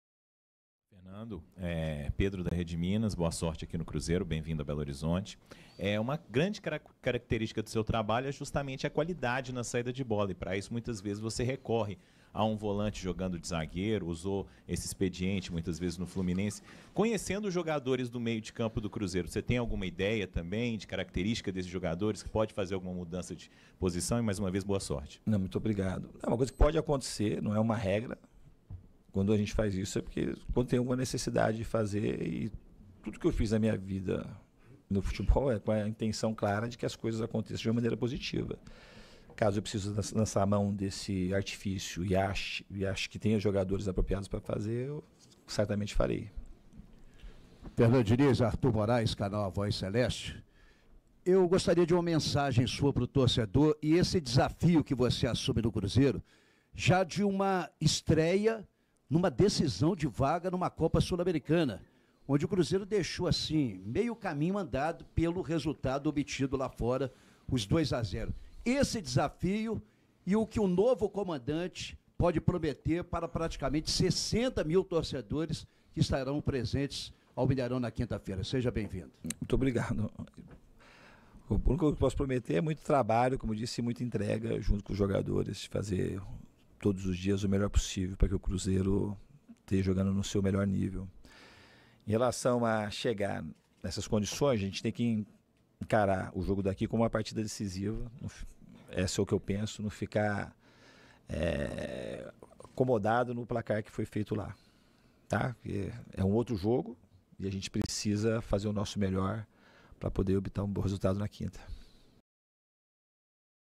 Sem acomodação com a vantagem, Fernando Diniz promete entrega total por mais uma vitória, na quinta!
O que você acha, torcedor? Deixe sua opinião.

Balanço Celeste de segunda a sexta.

 • BALANÇO CELESTE

KTO - ONDE A DIVERSÃO ACONTECE: Use o Cupom CELESTE e ganhe um bônus de primeira recarga.